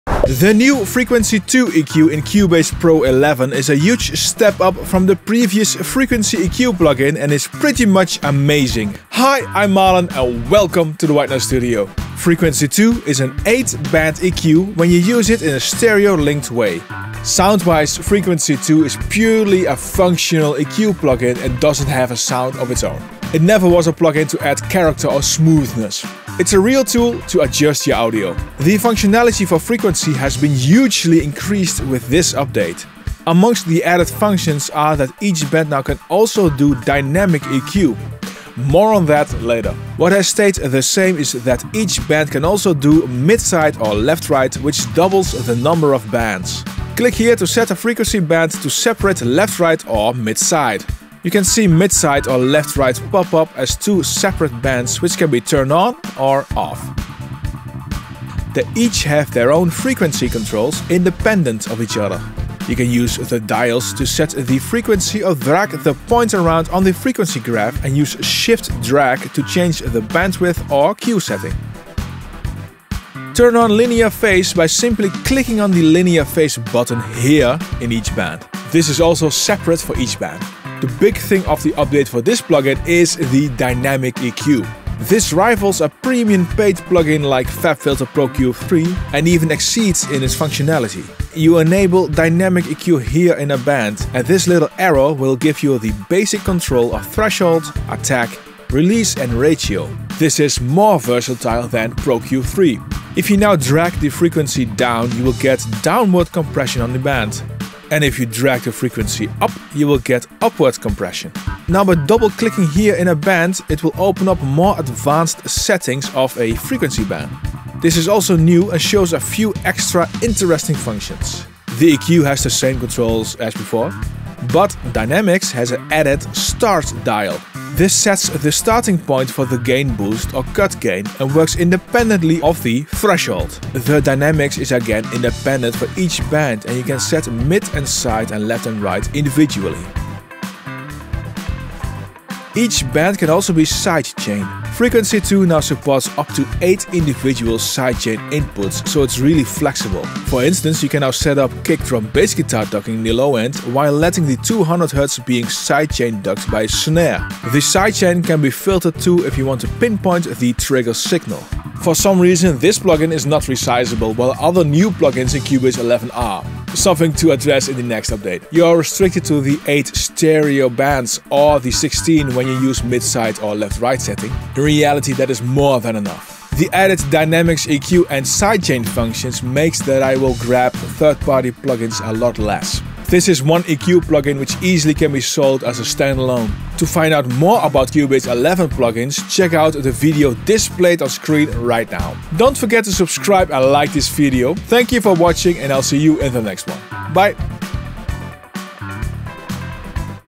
Frequency 2 - Amazing New Cubase Pro 11 and Nuendo 11 Dynamic EQ plugin!
The amazing brand new Frequency 2 in Cubase PRO 11 and Nuendo 11 now features Dynamic EQ which can rival and even surpass Fabfilter Pro-Q3

00:00 Intro
00:25 The added new functions - dynamic eq
00:40 How to use frequency 2
01:23 Using the dunamic EQ of Steinberg Cubase Frequency 2
02:24 Sidechain support

Thanks!
Marlon